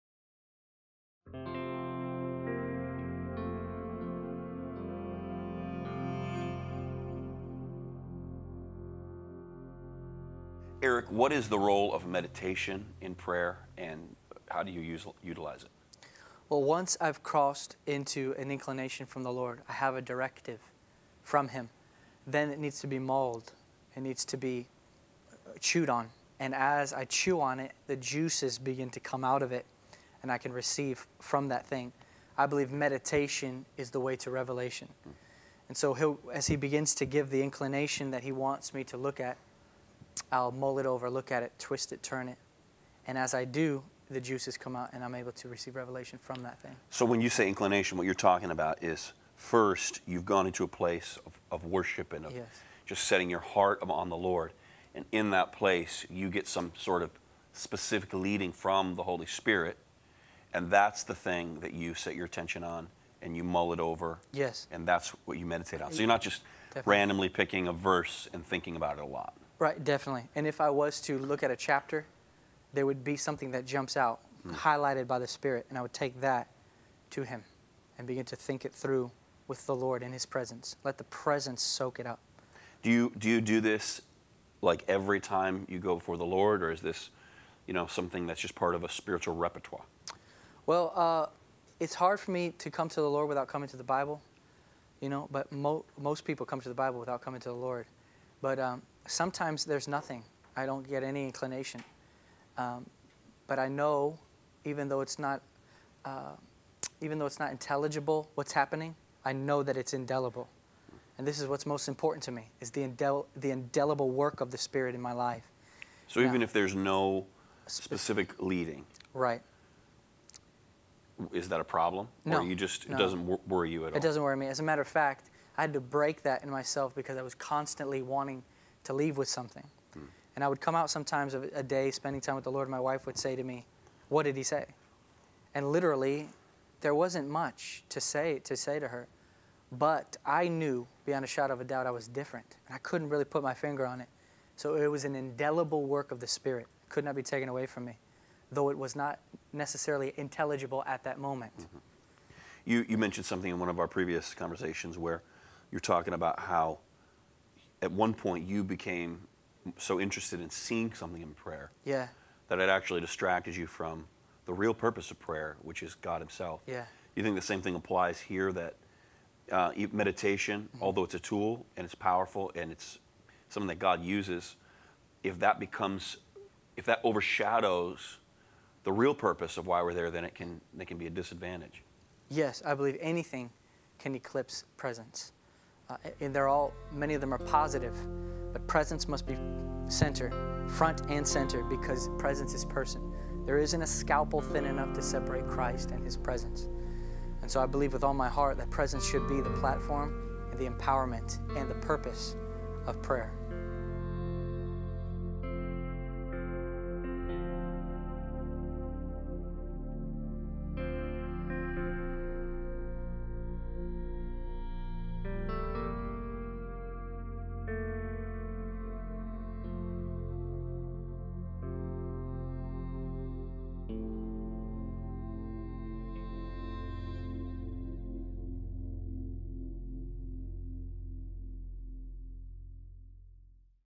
#23 - Meditation in Prayer
Eric Gilmour and Daniel Kolenda discuss the Biblical practice of meditation and it's proper place in our prayer lives.

Facebook.com/sonshipINTL